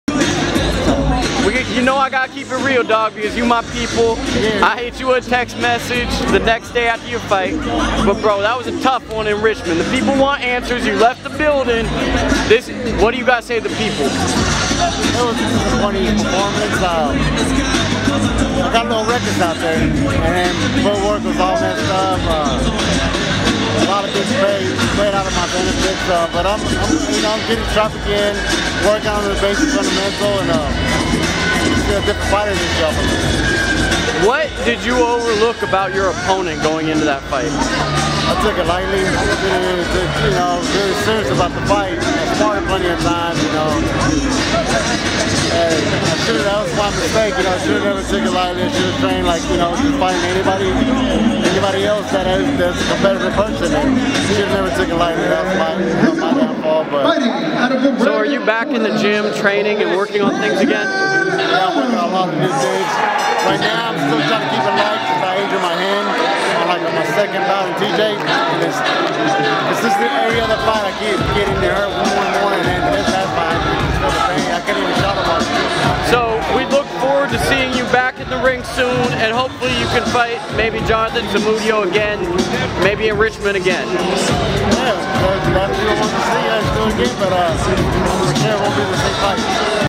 Luis Garcia "If I face Zumdio again, I won't be the same fighter"
Luis Garcia makes his first public appearance since fighting in his hometown of Richmond, CA. Garcia is hoping to get back to action soon.